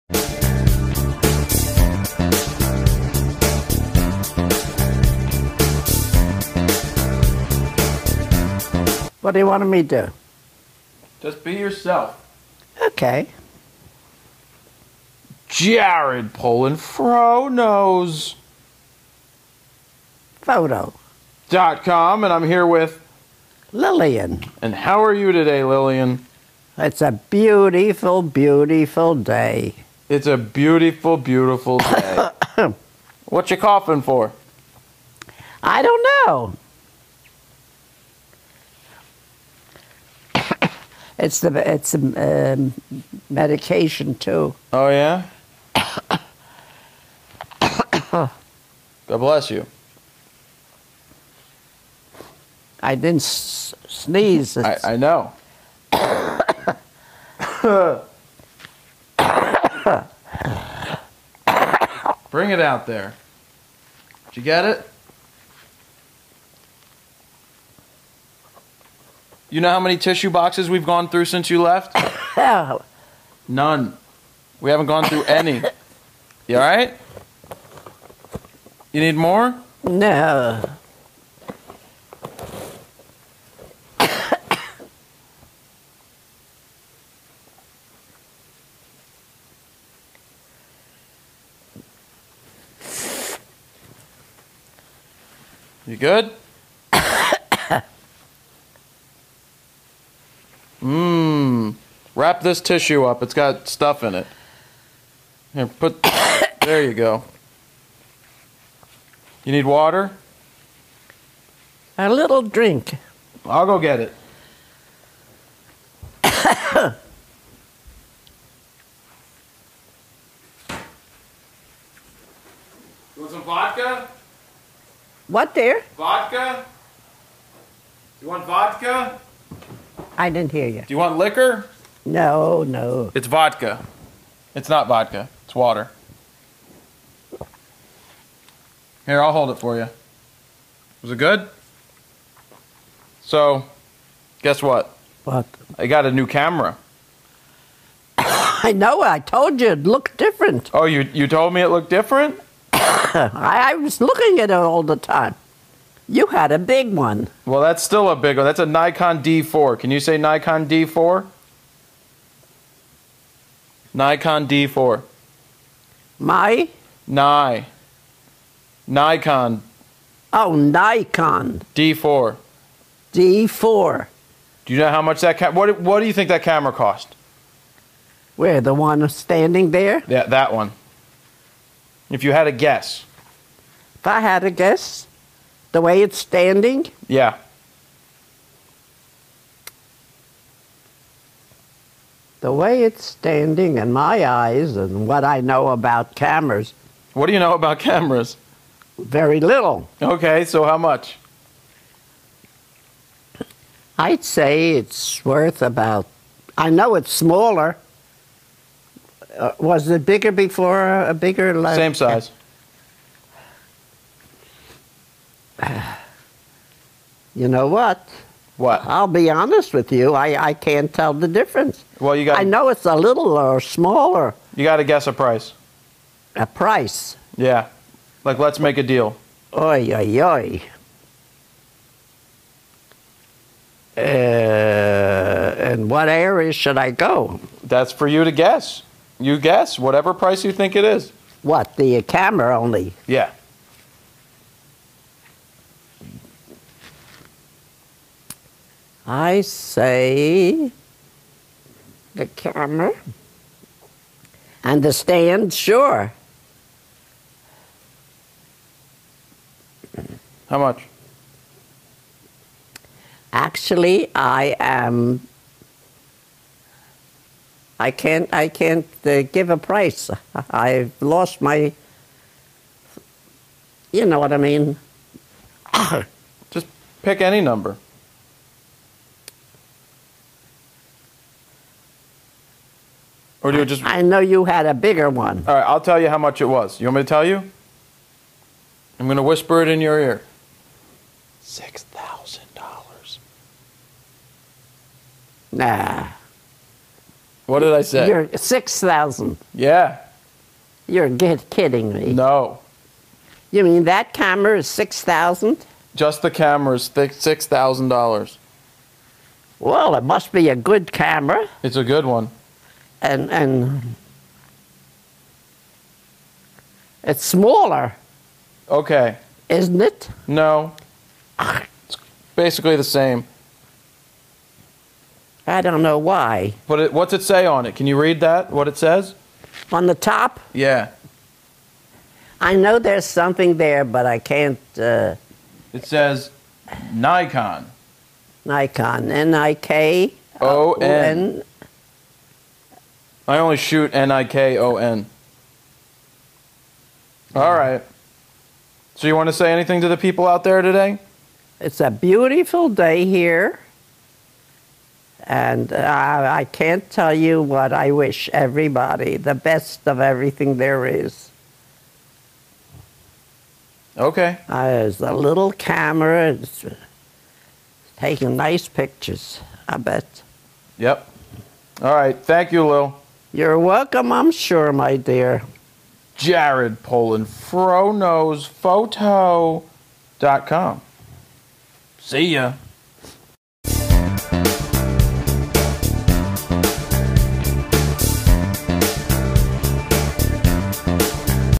The Fro and His Grandma Lil #91 - Coughing
Lil was over for a family dinner recently so we got to chatting on video again.  It's amazing how much she lights up when its time to make a video or two.

This time she got to coughing pretty strong so I had to go get her a cup of water.  She is starting to feel better though.  They say its like when a kid goes to daycare for the first time they pick up different sicknesses.  With Lil she really has not been exposed to that many people for over 20 years.